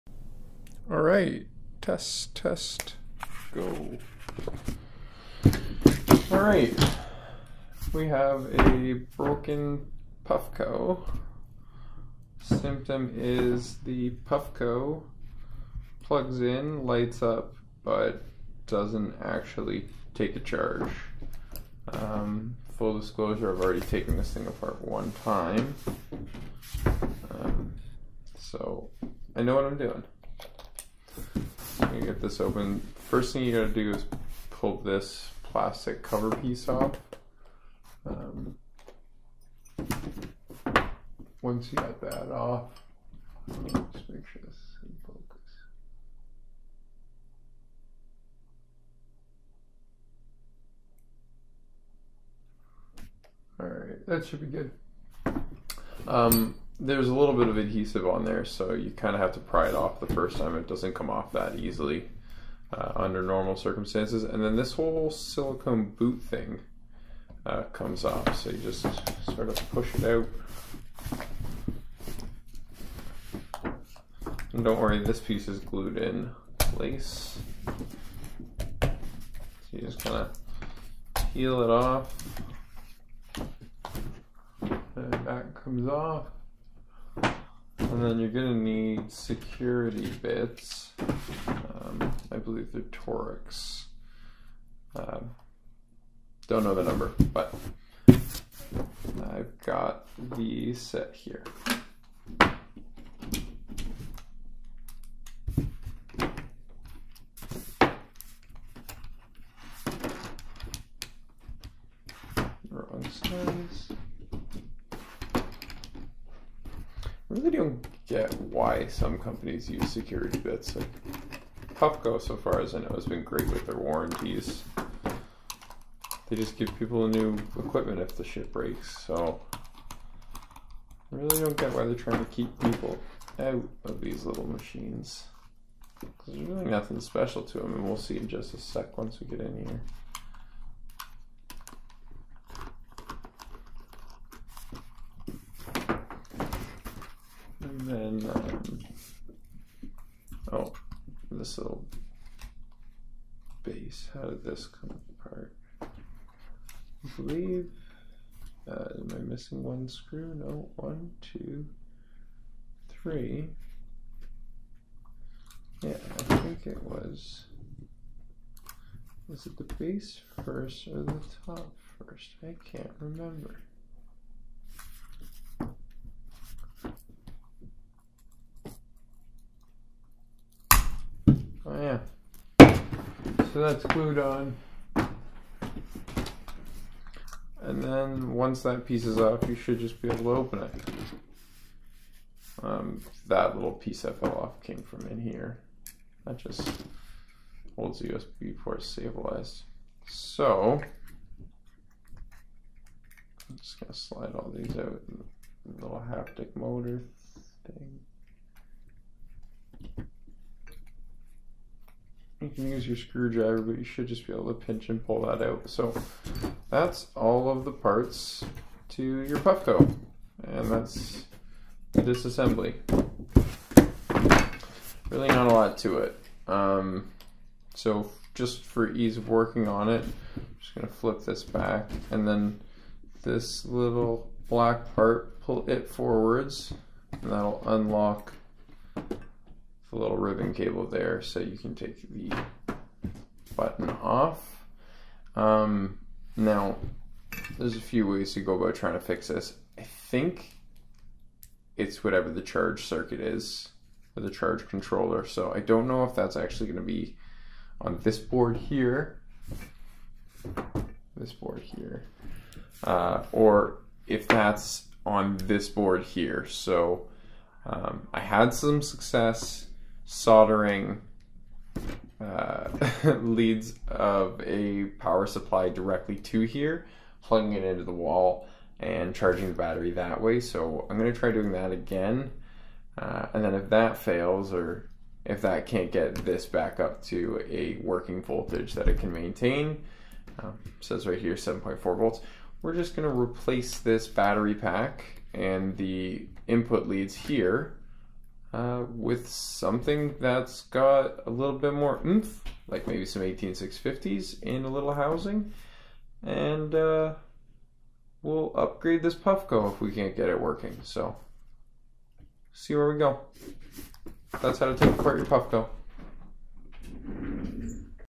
Inside of a Puffco - How to Open a Puffco Peak that Doesn't Charge Anymore
A friend of mine had a defective Puffco Peak, that would no longer charge. I show you how to open up the case and disassemble your vaporizer.
Puffco Quartz bucket
New Puffco
Check out my next video, where we solder on some leads and jump start our Puffco battery!